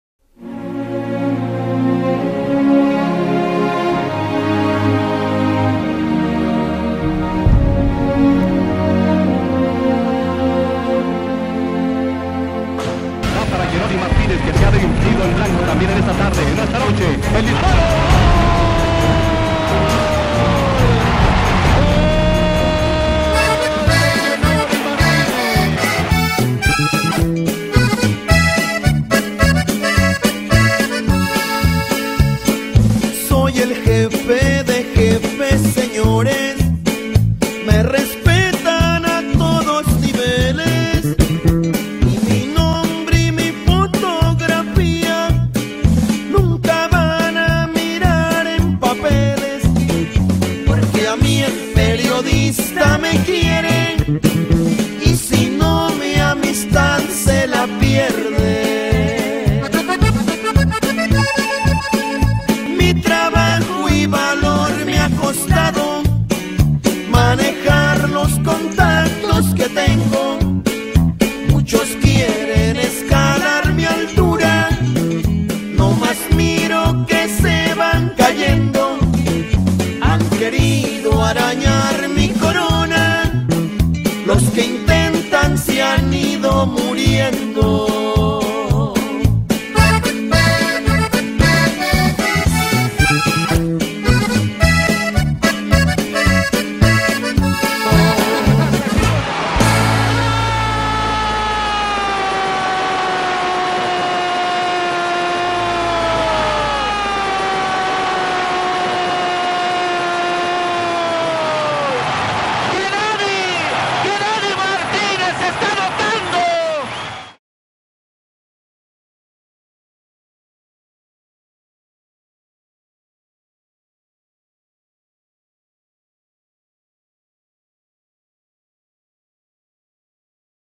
Genoni Martínez 600 partidos
Genoni Martínez llega a 600 partidos como profesional del Futbol Rápido y los cumple jugando para el Monterrey Flash